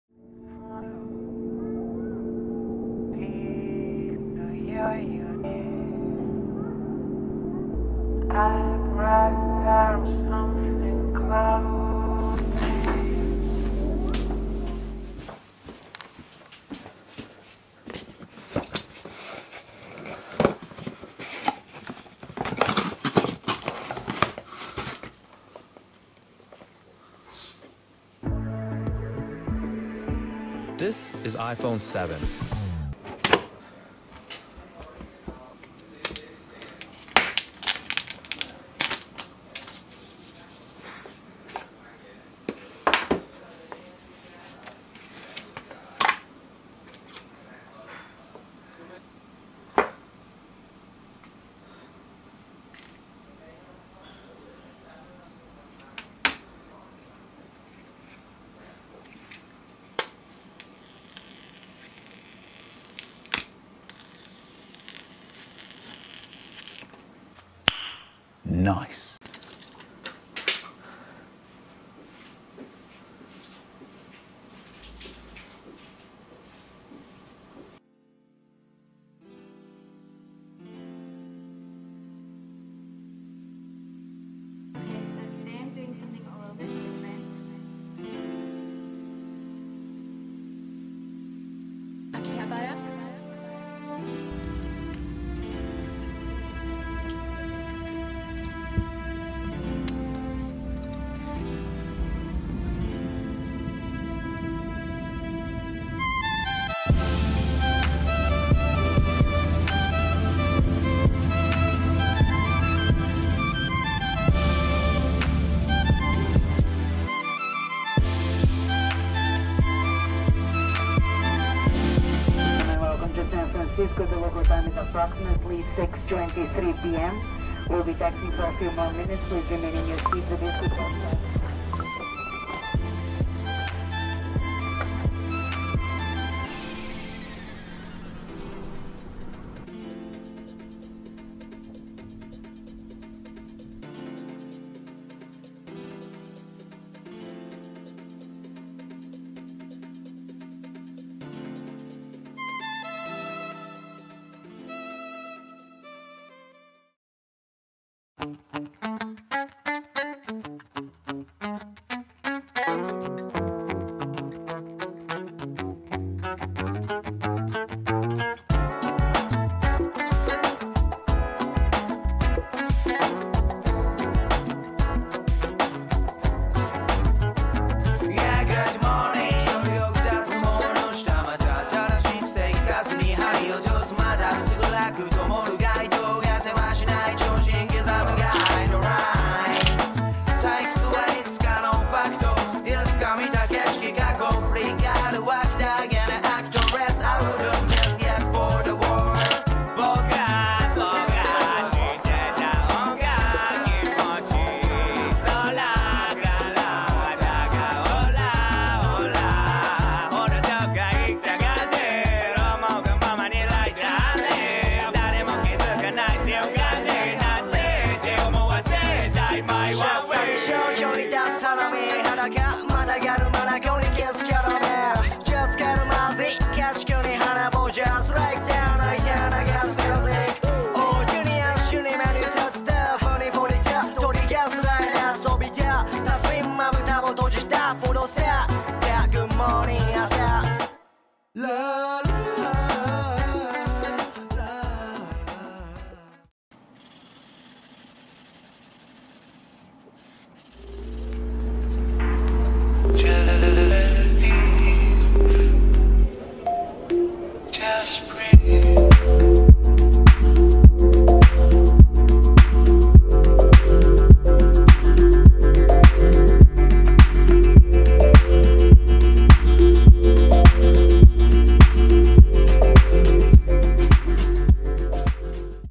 Jet Black iPhone 7 Plus and San Francisco | Thuc Phan
Just a quick compilation of my weekend in San Francisco. Didn't record too much, mostly just pictures. But that sunrise sure is amazing though.

**Most of the video was shot on the iPhone 7 Plus.